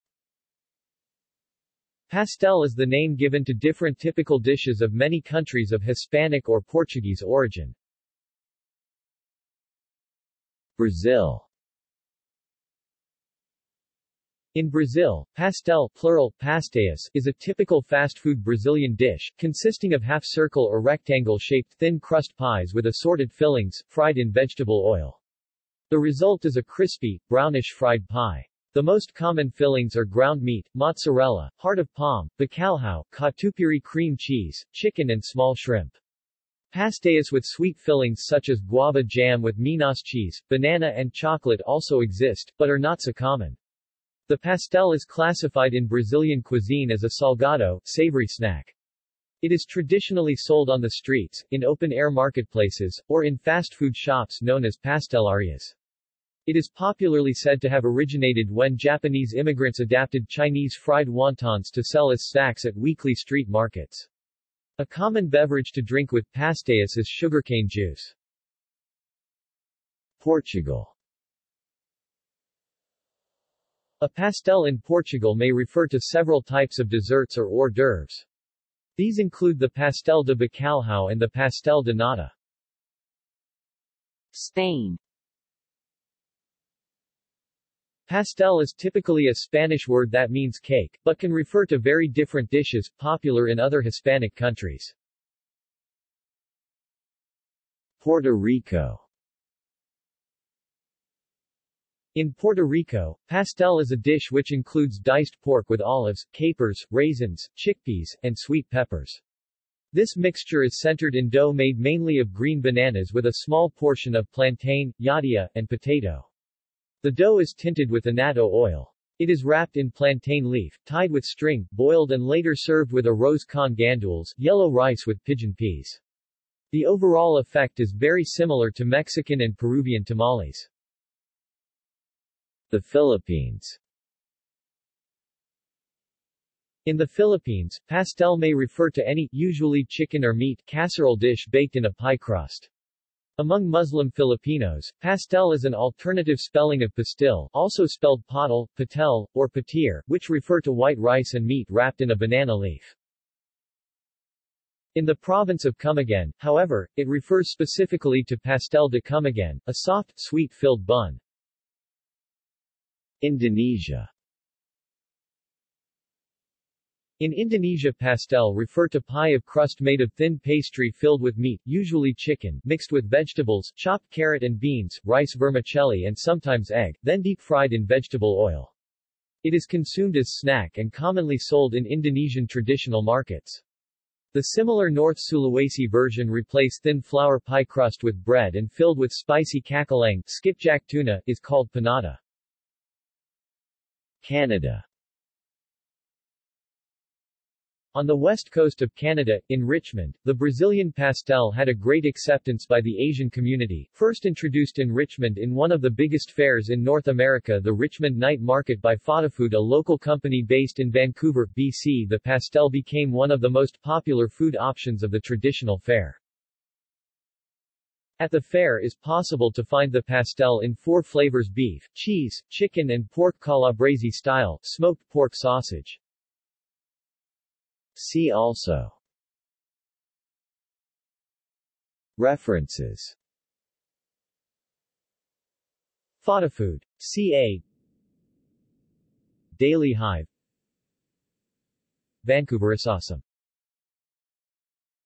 Pastel (food) | Wikipedia audio article
This is an audio version of the Wikipedia Article:
Pastel (food)

SUMMARY
Pastel is the name given to different typical dishes of many countries of Hispanic or Portuguese origin.